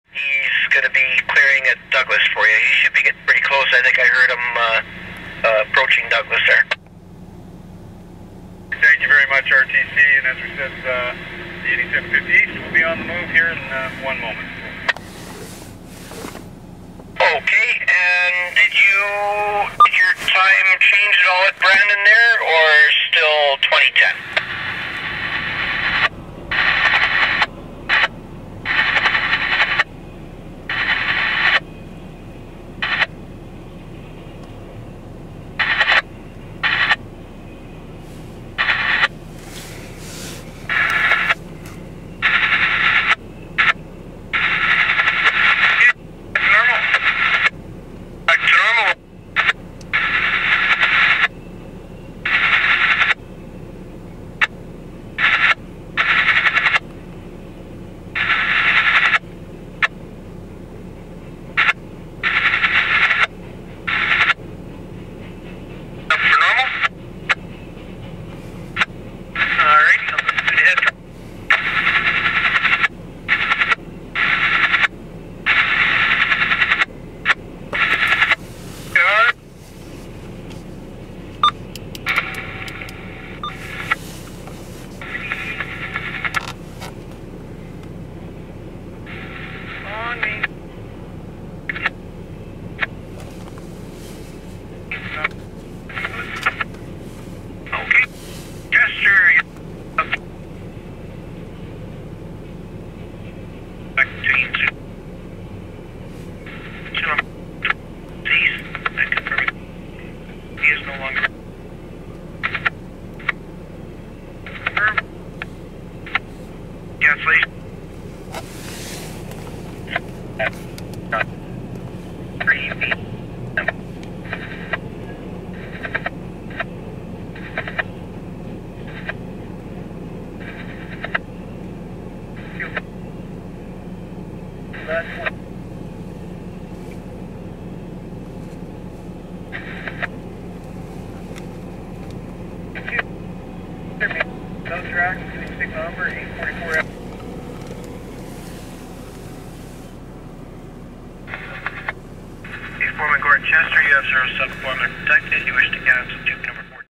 Train During 2015 Teamsters Strike! - CP Carberry Sub at Brandon East
An eastbound train is seen being operated during the 2015 strike the the Teamsters. I'm not really a fan of this practice.